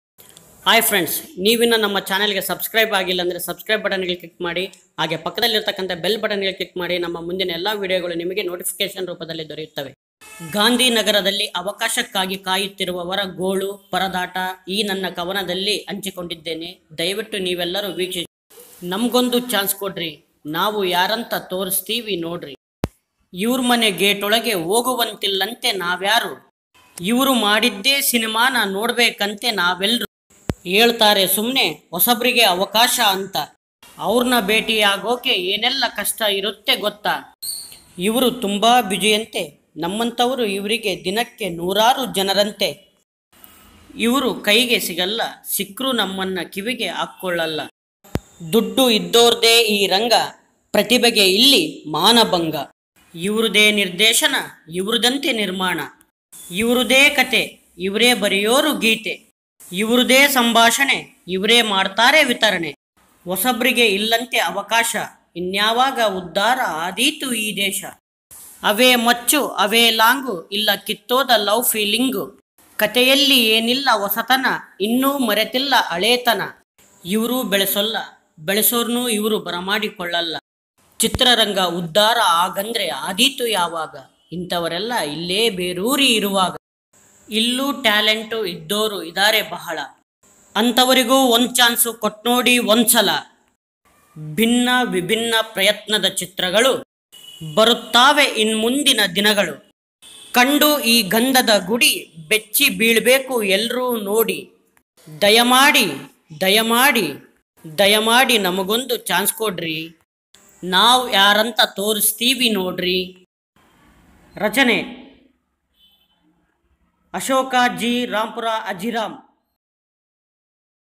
ಗಾಂಧೀನಗರದಲ್ಲಿ ಅವಕಾಶ ಬೇಕಾ?
ಗಾಂಧೀ ನಗರದಲ್ಲಿ ಅವಕಾಶ ಬೇಕಾ?  ಅವಕಾಶಕ್ಕಾಗಿ ಪರದಾಡುವವರ ಗೋಳು ಪರದಾಟ ಅಜಿರಾಂ ಕವನದಲ್ಲಿ